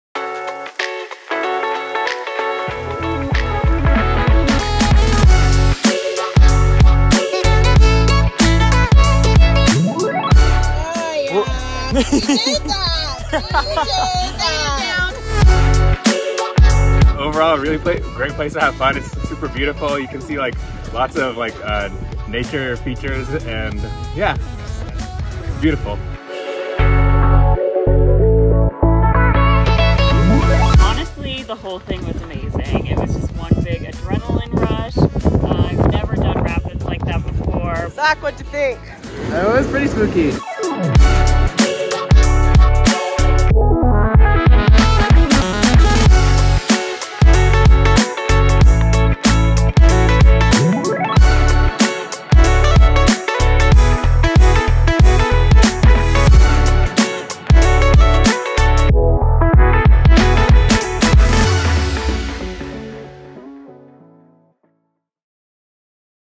All Smiles on Goodwin Canyon, October 15-16
Our talented river guides not only expertly led their crews through the exciting whitewater that Goodwin Canyon offers, they also captured the essence of this past fun-filled weekend on camera. And it was all thumbs up!